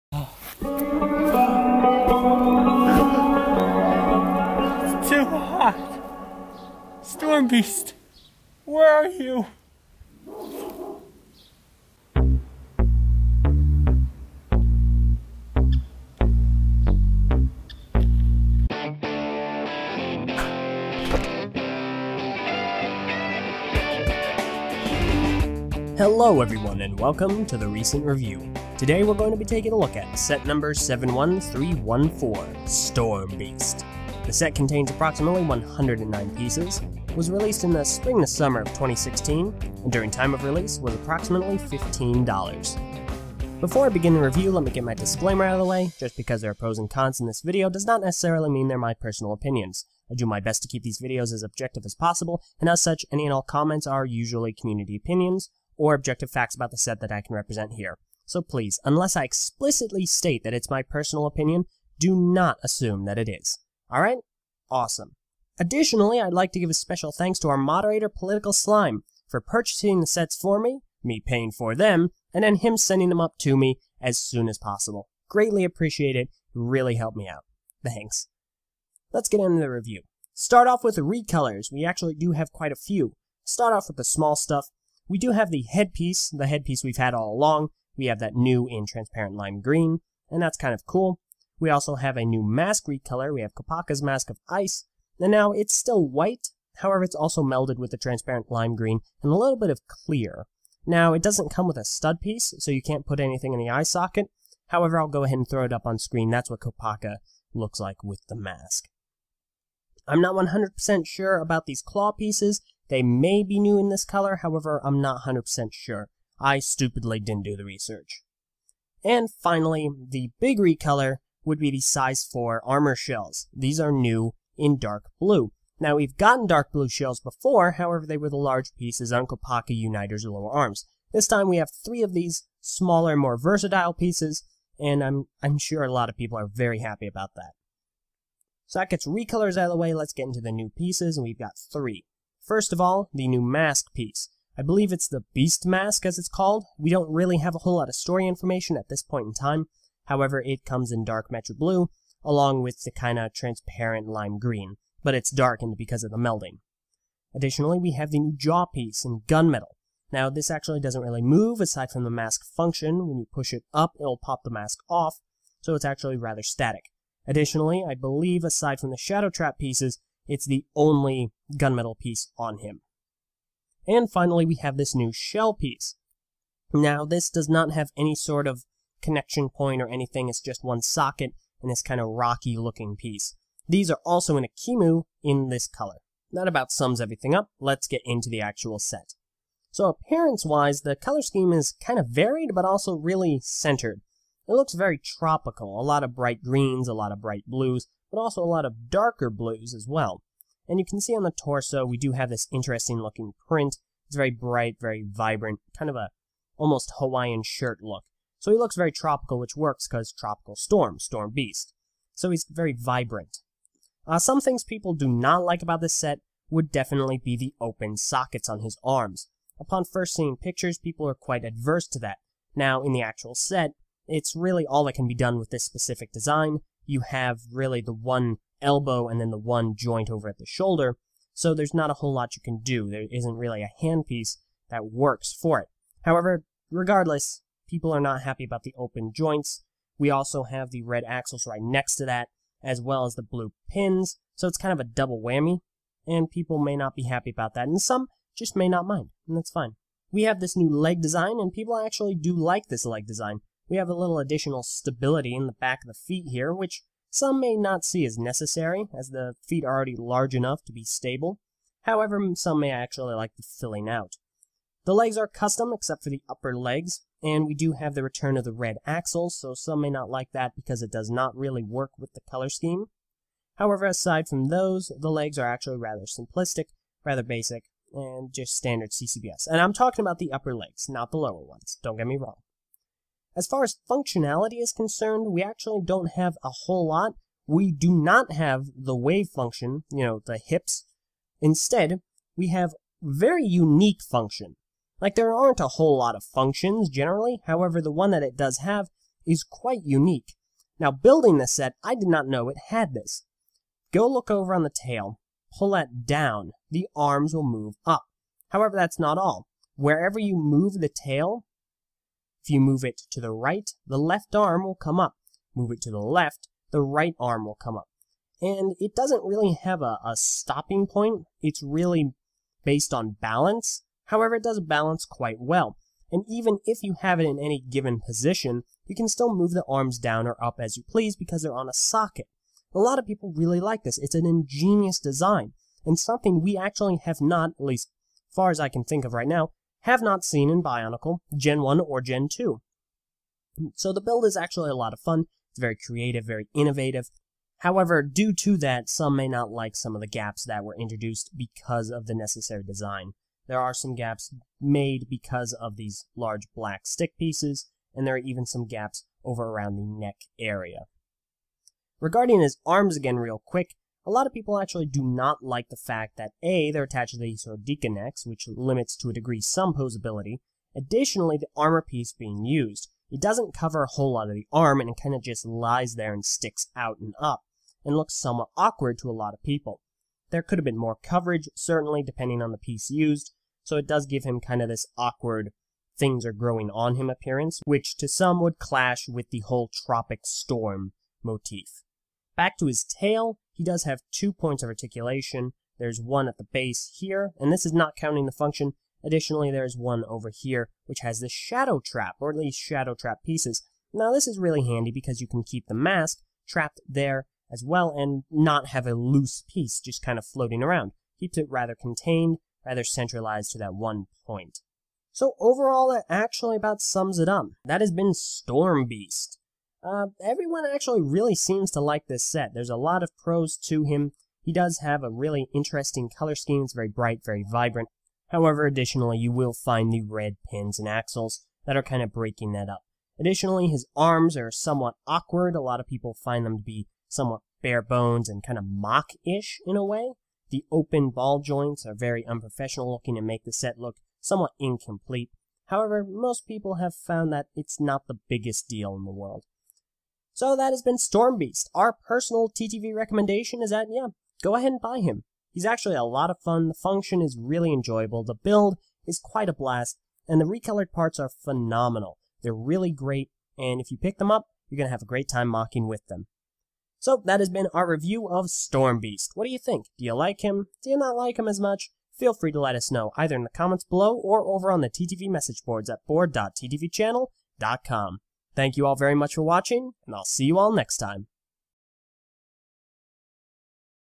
Eljay's Recent Review: 71314 Storm Beast
This week, let the plastic relax and Eljay suffer - It's time for Storm Beast!